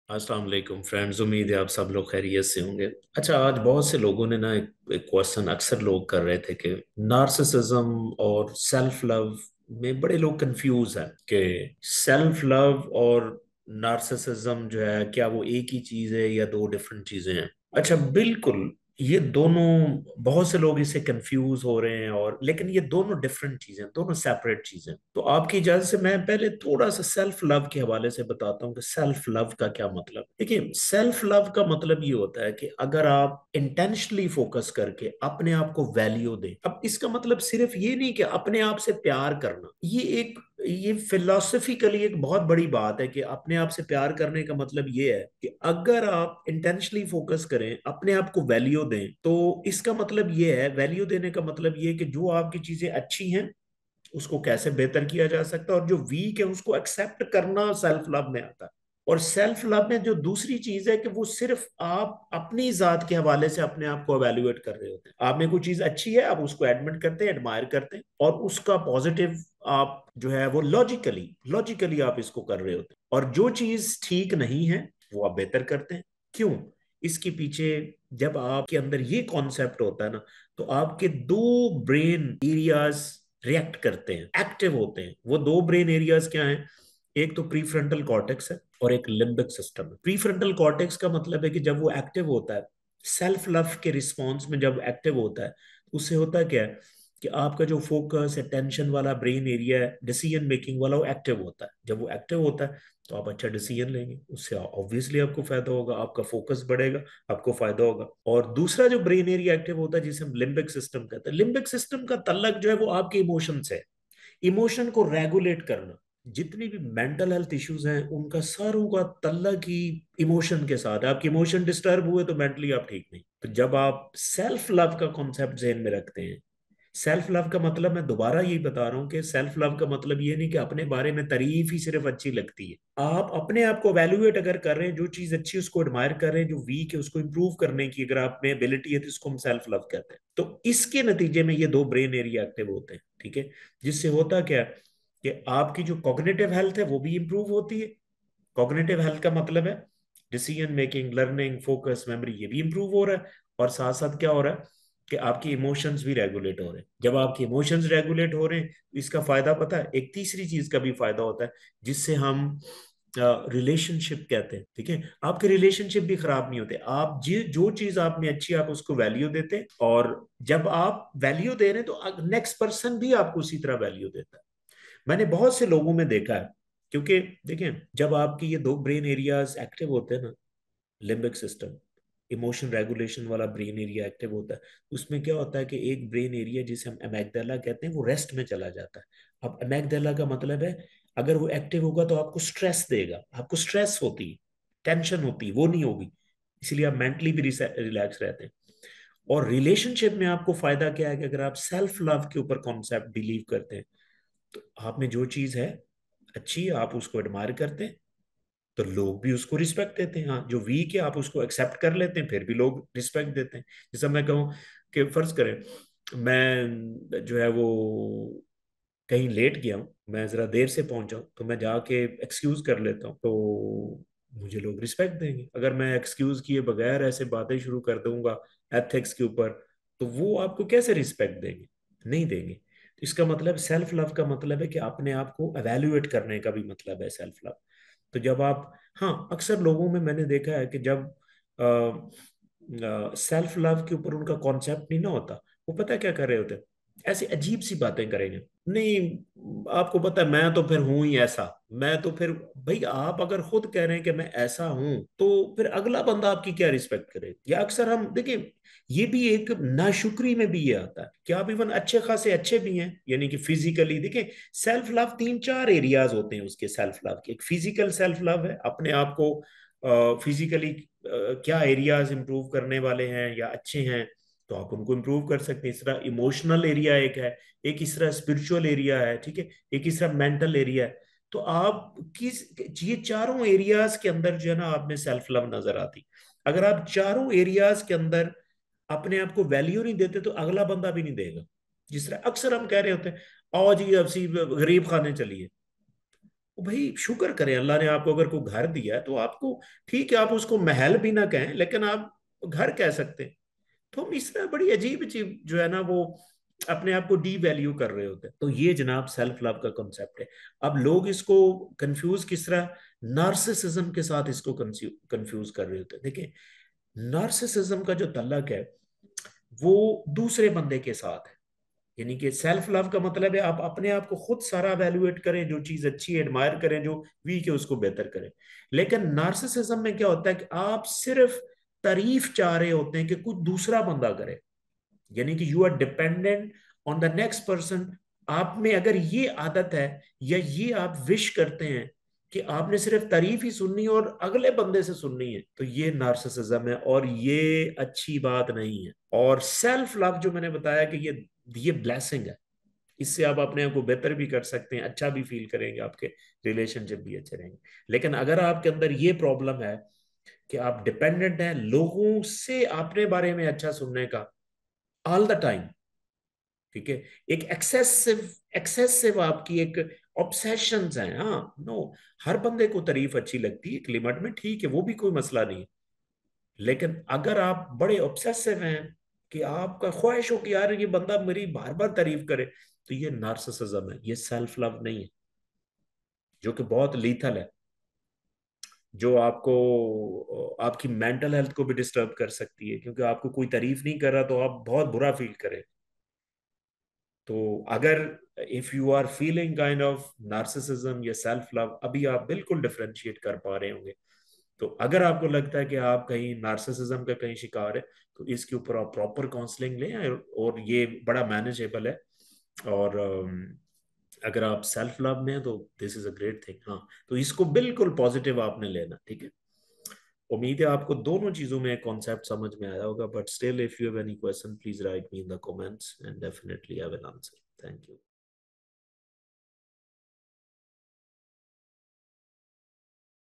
Difference between Narcissism and Self Love l Dr Rafiq Dar
Difference between Narcissism and Self Love
Dr Rafiq Dar
Corporate Trainer
Neuropsychologist, Hypnotherapist and NLP Practitioner